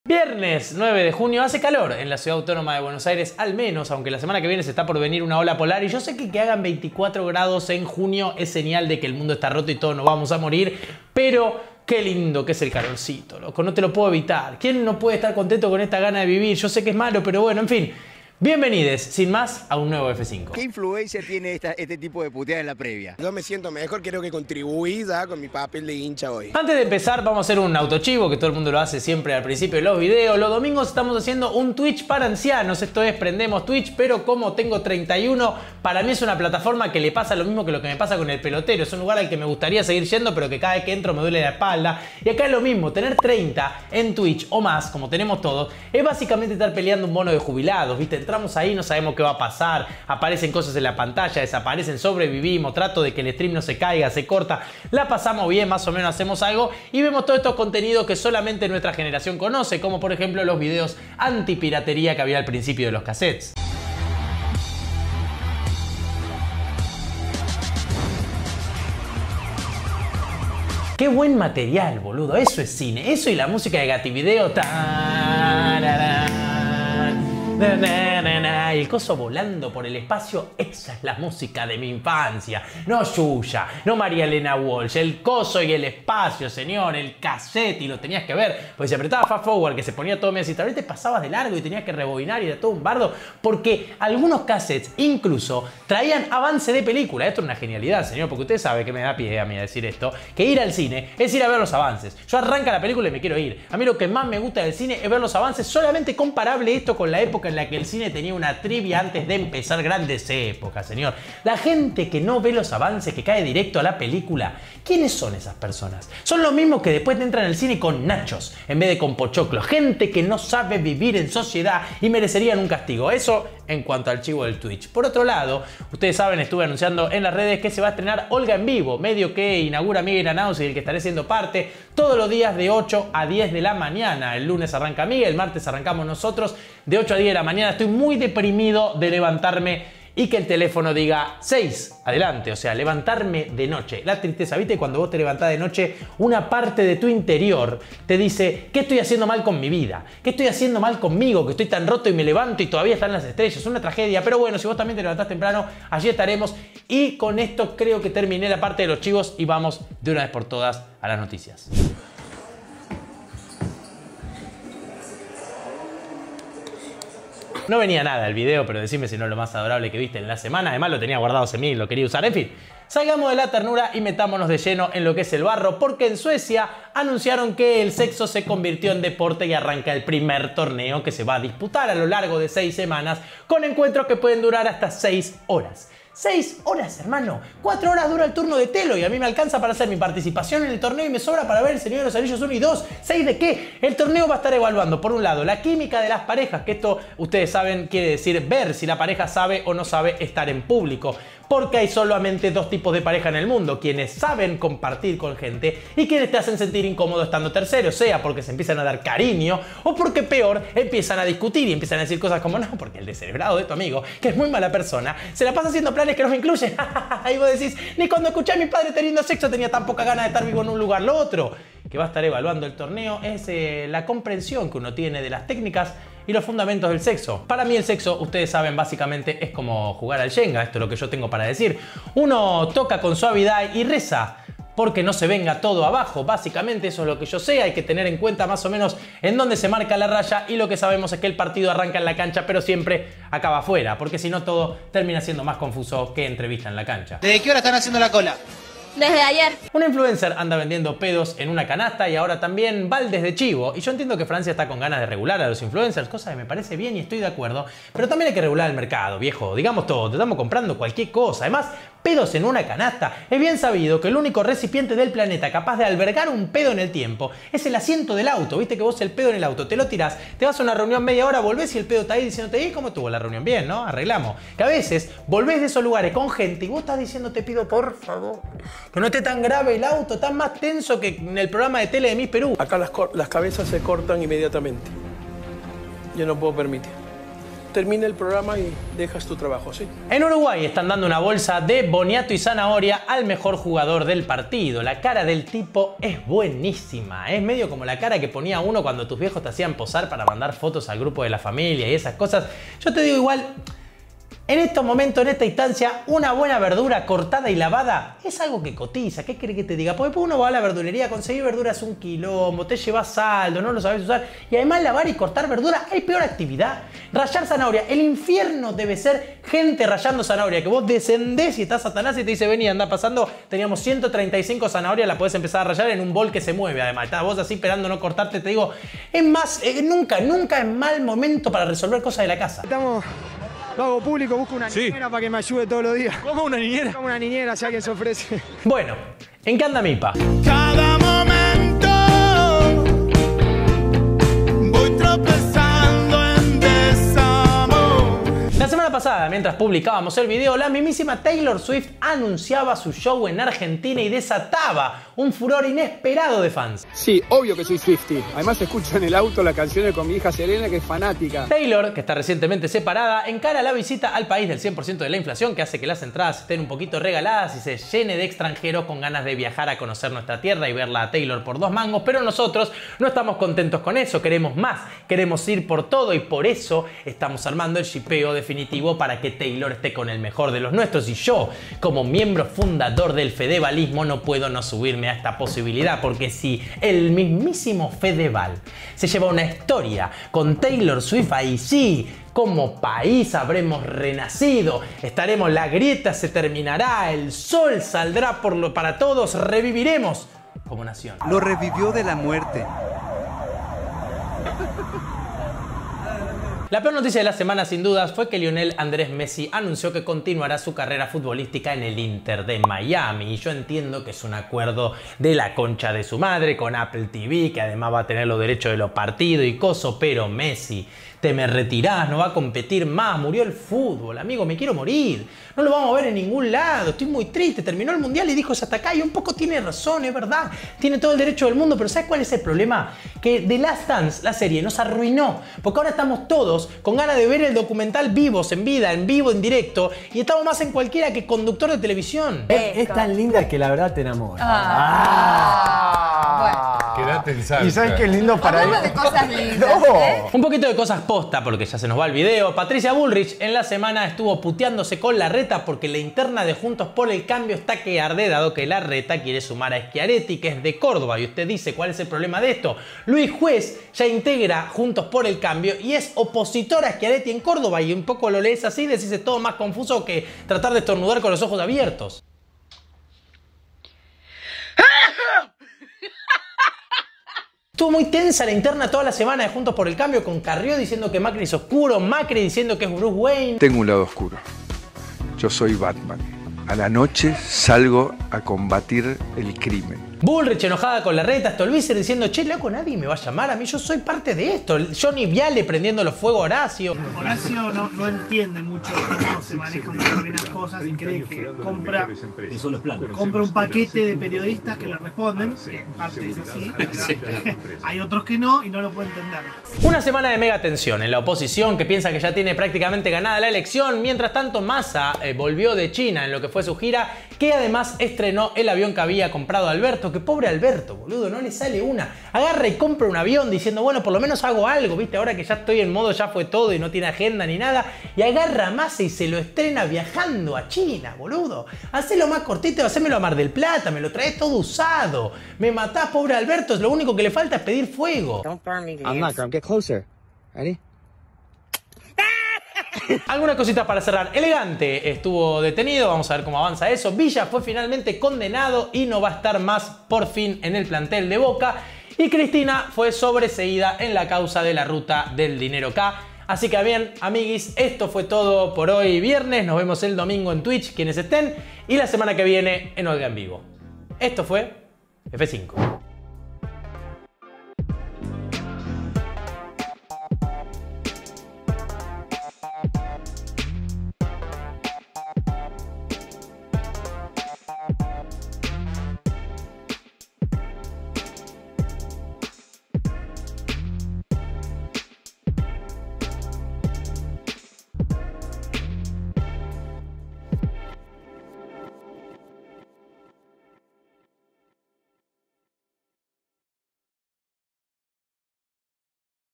F5 - CURADURÍA DE INTERNET: CÓMO COMPETIR EN EL MUNDIAL DE SEXO Y LAS INTERNAS
Mitad de año y la semana en internet
- Auto chivo
- El mundial de sexo
- Las ventas de influencers
- Las internas de JxC
- Messi a la MLS

y más

Toda la gente que hace posible esta movida:

Cámara, edición y memes: @rominarezi

Tapa diseño y amor: @naturalrois y @michupolansky

Producción y coso: @martodanz

Música original: @lucaselfire

Asistencia en las sombras: @gerardolipszyc